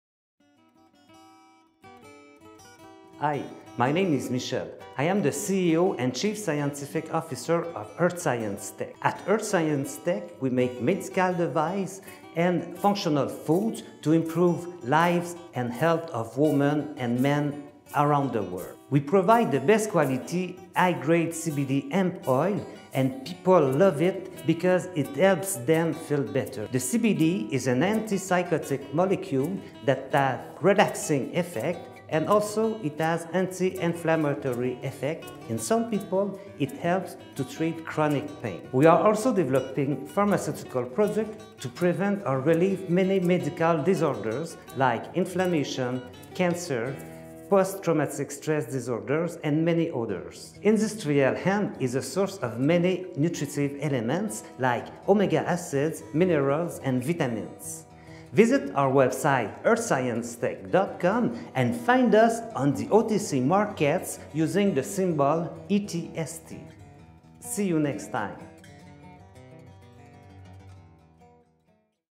Earth Science Tech
La voix du succès est fier de vous présenter Michel Aubé PhD
CEO & CSO
Michel Aubé PhD Expert  en Immunologie, Physiologie et Endocrinologie
Dr Michel Aubé a une expertise diversifiée dans les sciences de vie...